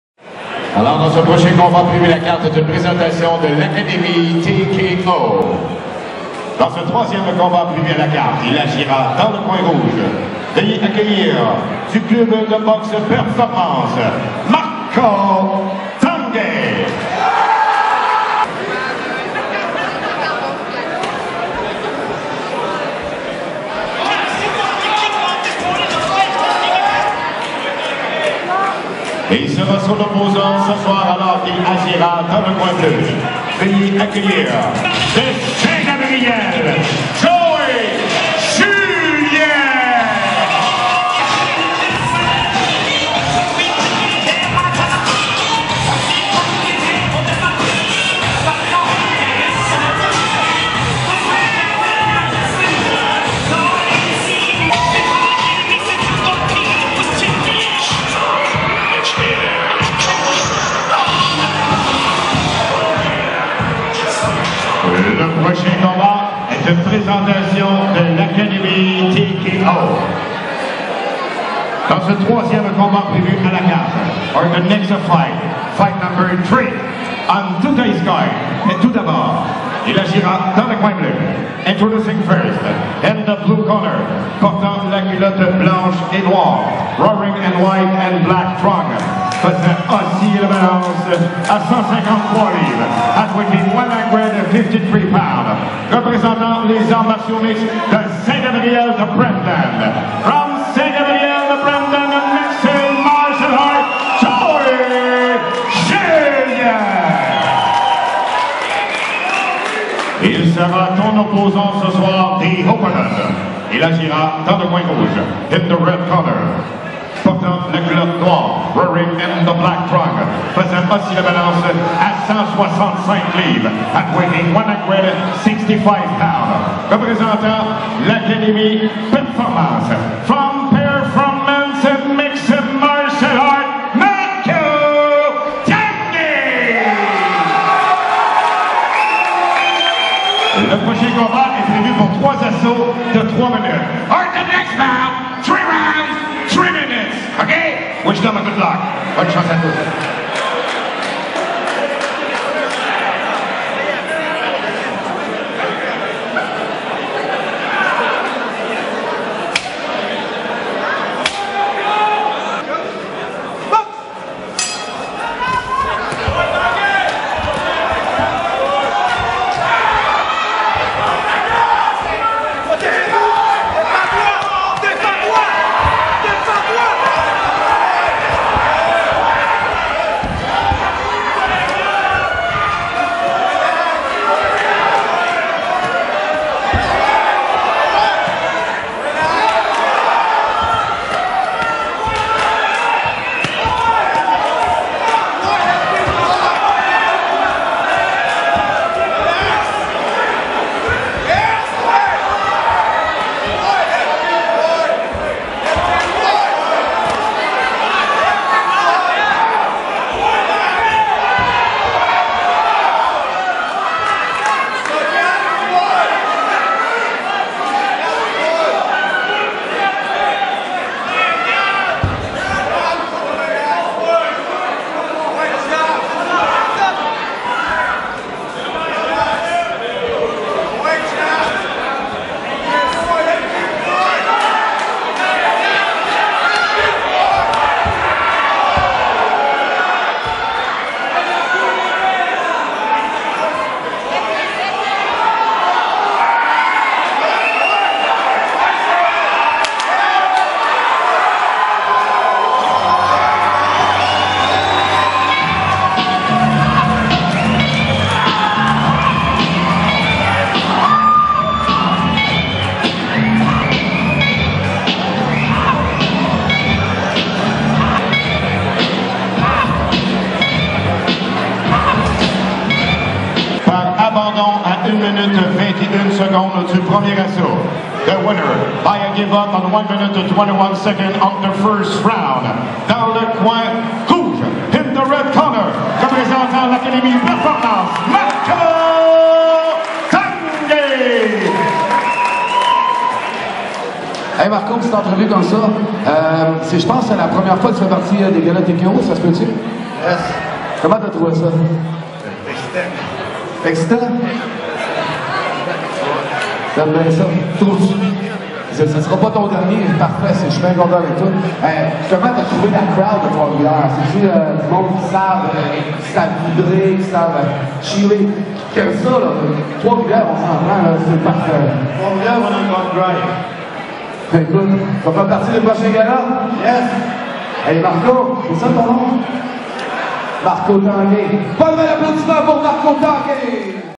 Marco Tanguay VS Joey Julien
Marco Tanguay affronte Joey Julien en combat MMA amateur, lors du GALA TKO 9 tenu a Trois Rivières, Québec, Canada le 5 ocrobre 2013. Gracieuseté de Gabriel Lavallée.